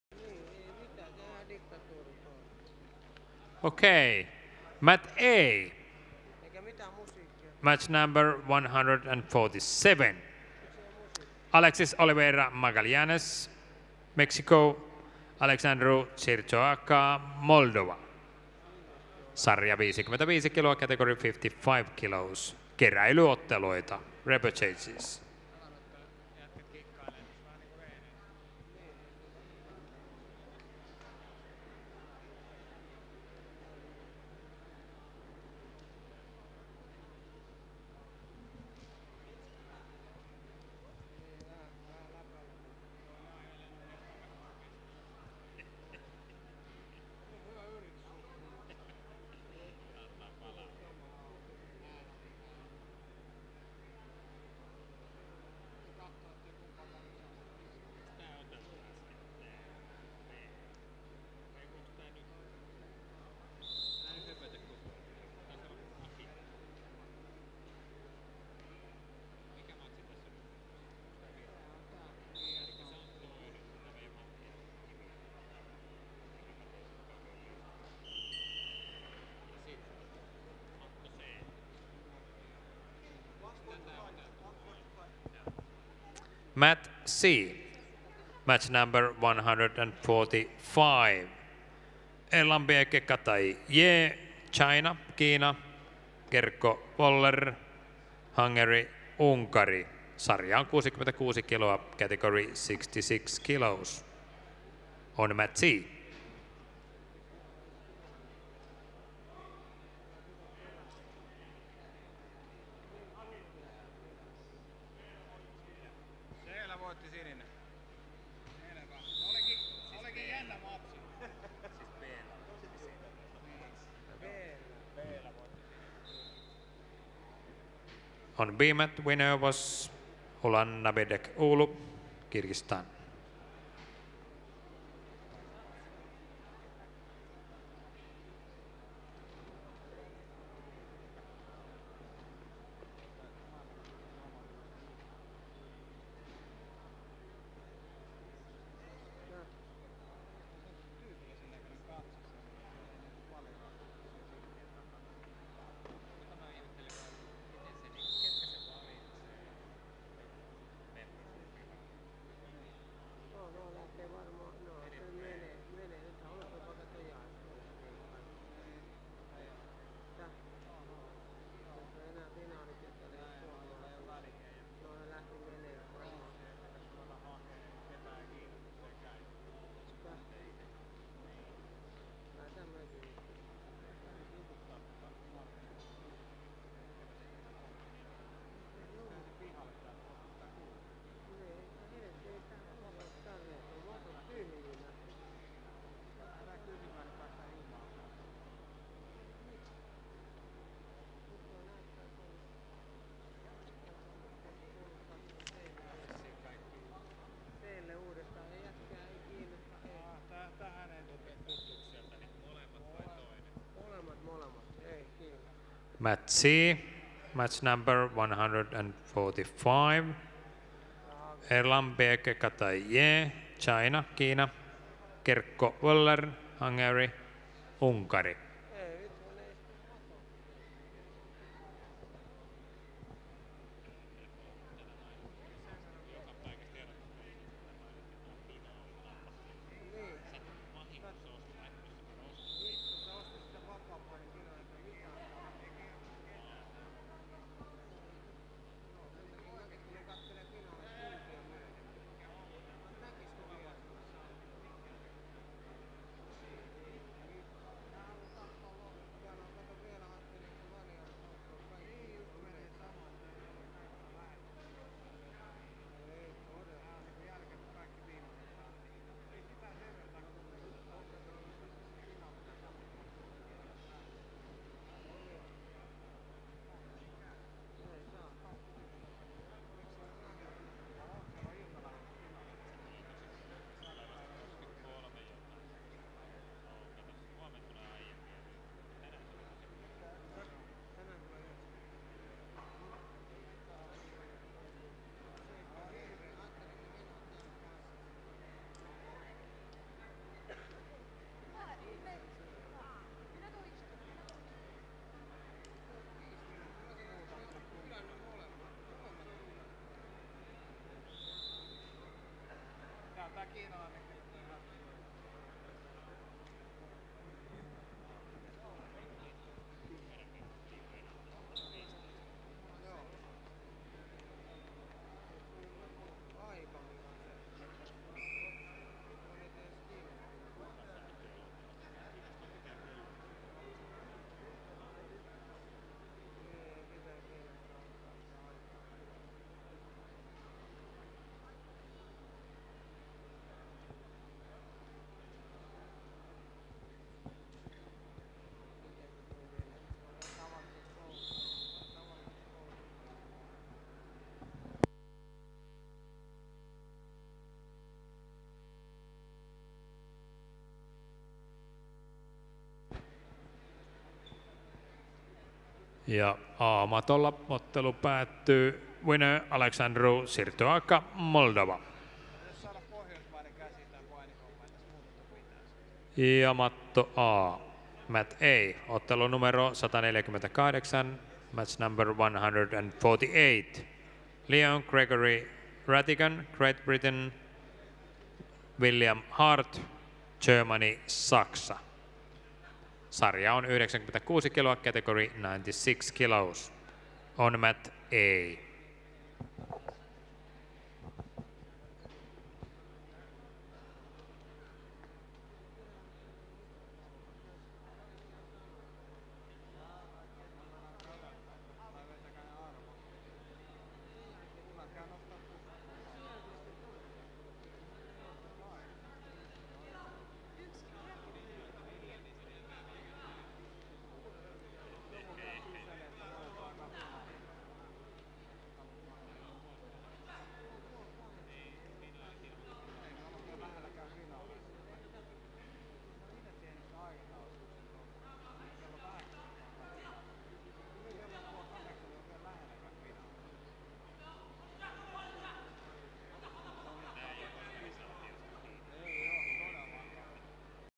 paini 040512 kuvaus 6.mp4
2nd World Qualifying Tournament on Friday 4th May 2012 in Vantaa Finland. Qualifier. Freestyle.